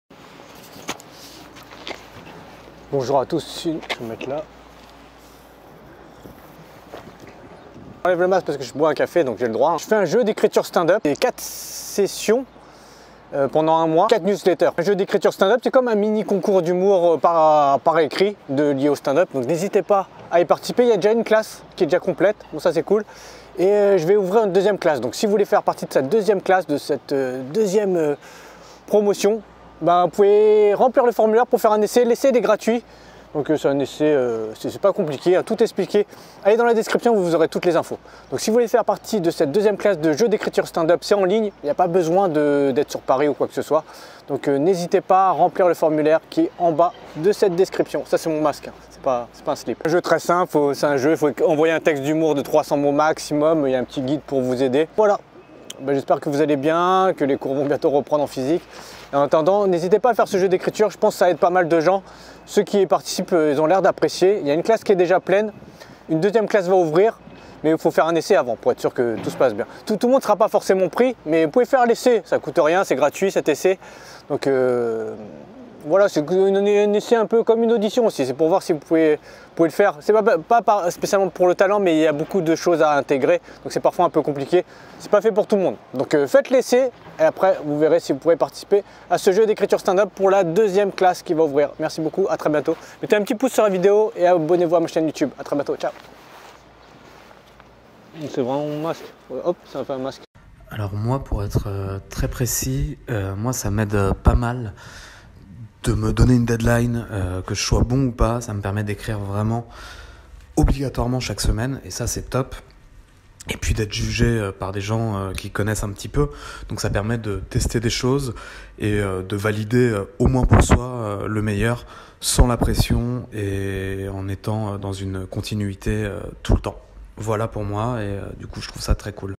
Nouvelle classe : jeu d'écriture stand up en ligne
Apprenez à écrire un sketch de stand up avec un mini jeu d'écriture à faire en ligne.